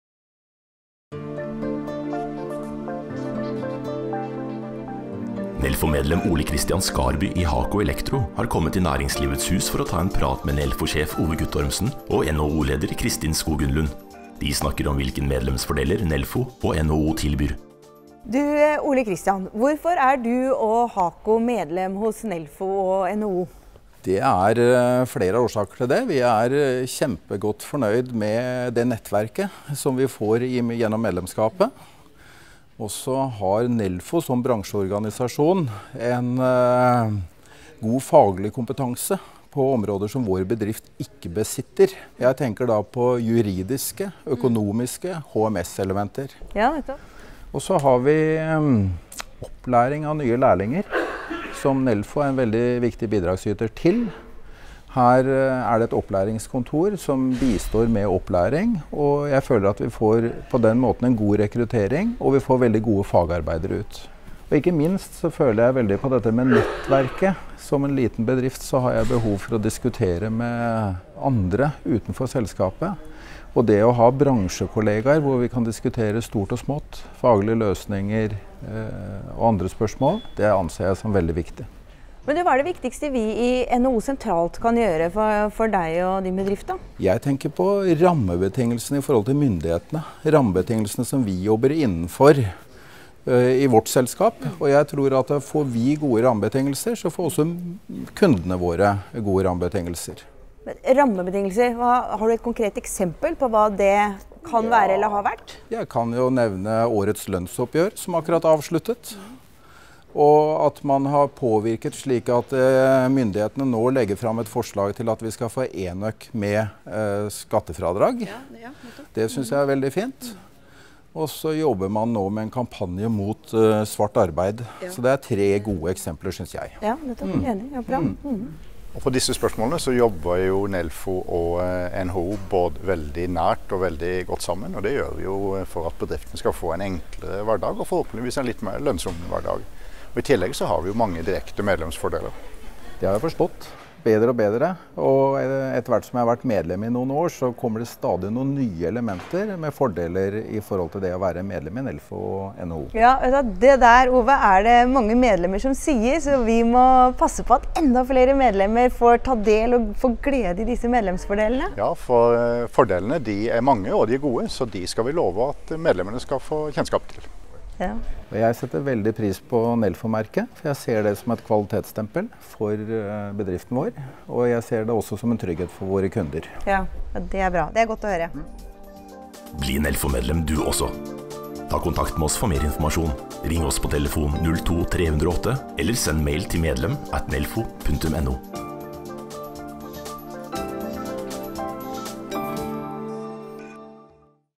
Medlemsfordeler i Nelfo og NHO
Daglig leder i Hako elektro forteller om fordelene ved å være medlem i Nelfo.